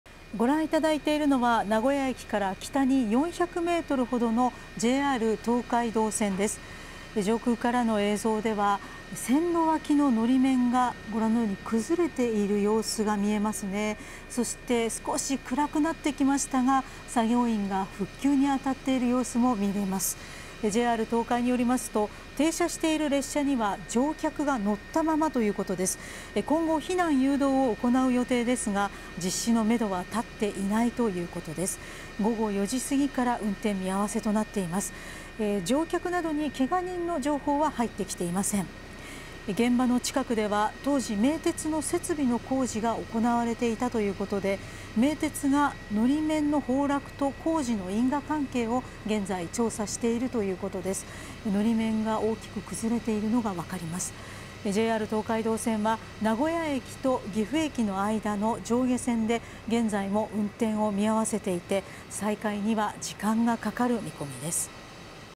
線路脇の法面崩れる…JR東海道線・名古屋駅-岐阜駅間の上下線で運転見合わせ 当時現場近くで名鉄の設備工事
JR東海道線では線路脇の法面が崩れ、12日午後4時すぎから、名古屋駅と岐阜駅の間の上下線で運転を見合わせています。

2024年3月12日放送